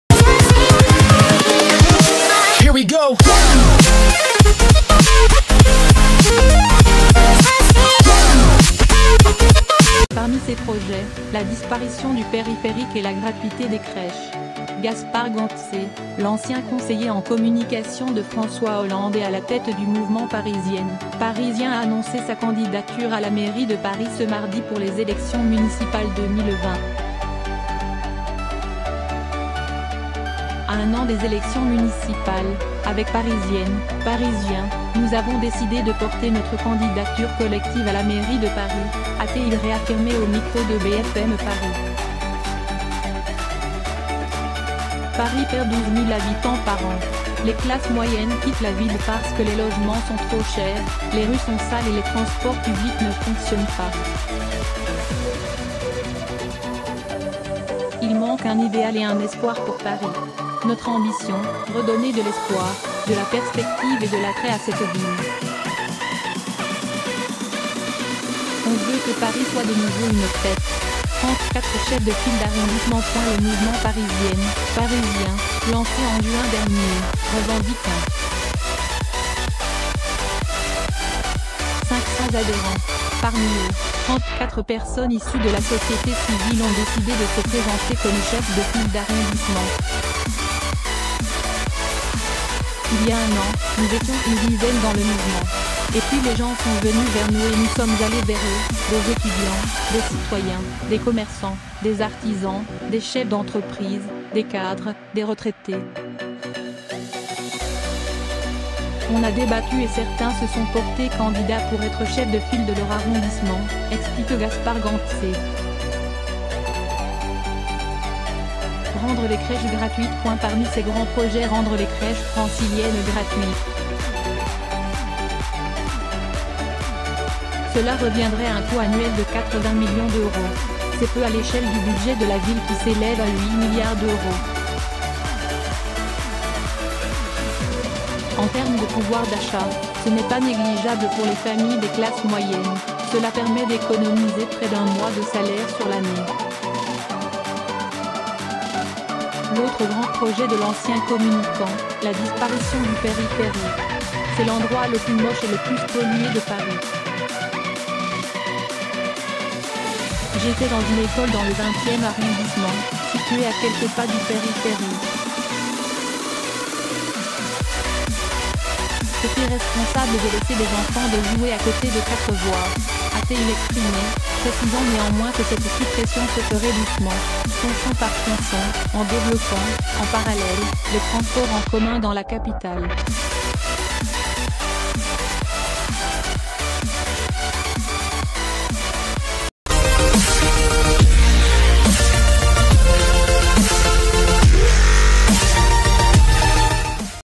Municipales: "On veut que Paris soit de nouveau une fête", Gaspard Gantzer détaille son programme
Parmi ses projets: la disparition du périphérique et la gratuité des crèches. Gaspard Gantzer, l'ancien conseiller en communication de François Hollande et à la tête du mouvement Parisiennes, Parisiens a annoncé sa candidature à la mairie de Paris ce mardi pour les élections municipales 2020."A un a...